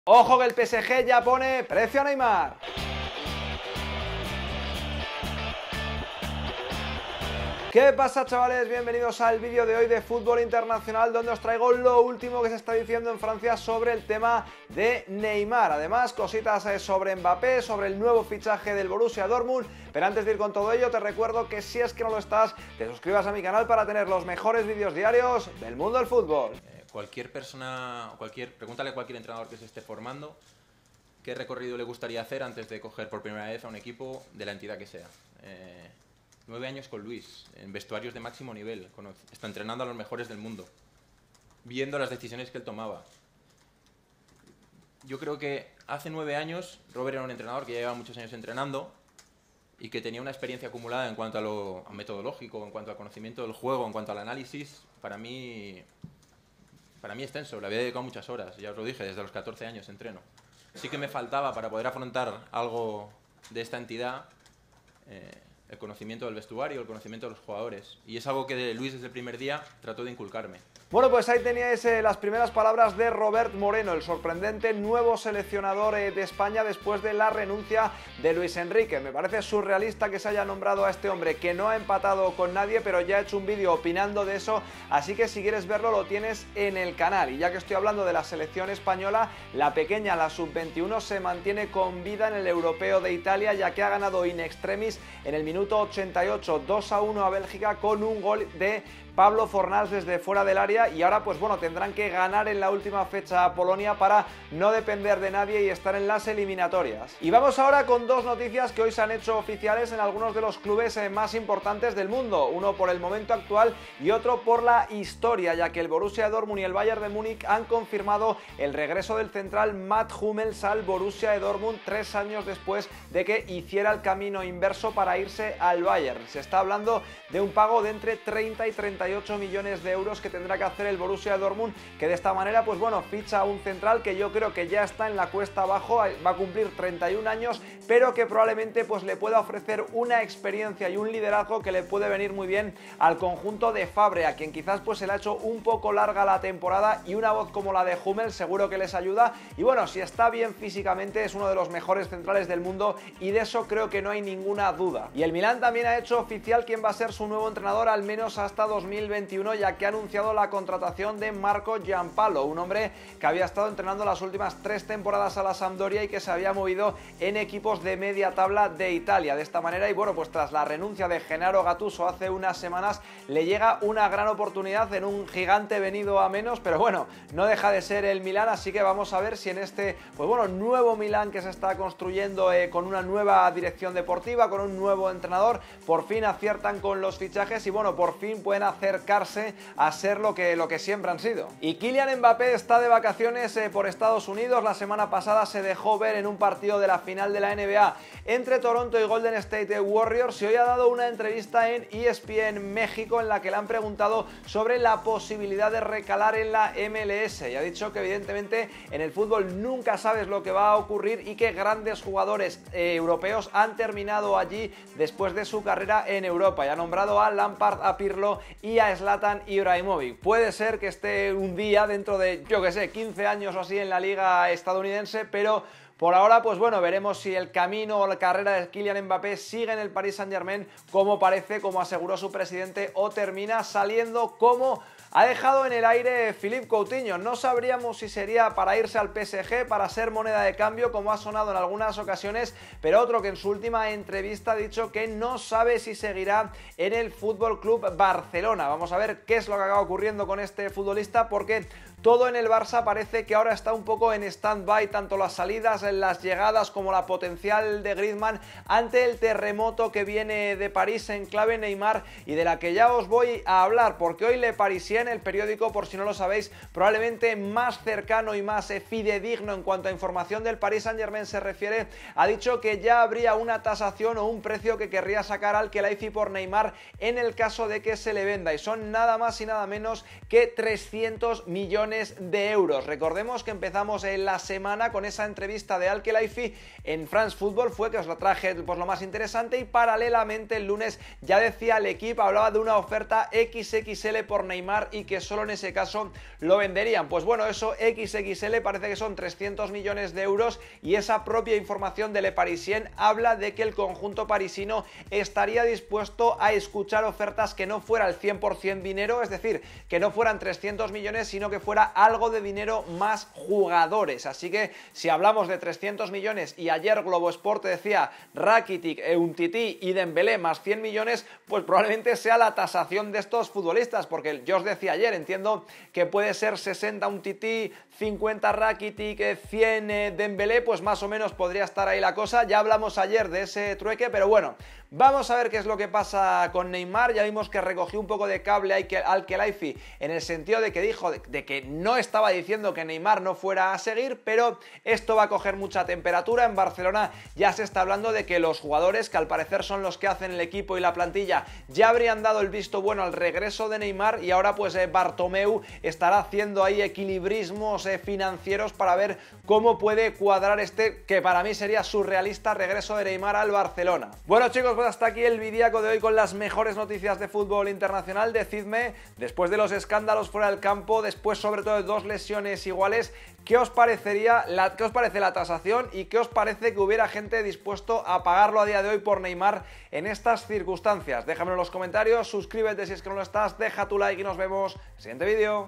EL PSG PONE PRECIO A NEYMAR | HUMMELS VUELVE BORUSSIA | NUEVO ENTRENADOR MILÁN | FUTURO MBAPPE
Noticias de fútbol protagonizadas por el precio que le habría puesto el PSG a Neymar de cara a su salida del PSG. Al Khelaifi quiere un traspaso de récord para que Neymar salga del PSG y probablemente fiche por el Barcelona. Mbappe habla en Estados Unidos sobre su futuro y la posibilidad de fichar por la MLS. Coutinho reconoce que se está pensando irse del Barcelona. El Borussia Dortmund hace oficial el fichaje de Matts Hummels y Marco Giampaolo es el nuevo entrenador del Milán. Robert Moreno es el nuevo seleccionador de España y sustituto de Luis Enrique y España sub 21 gana con gol de Fornals.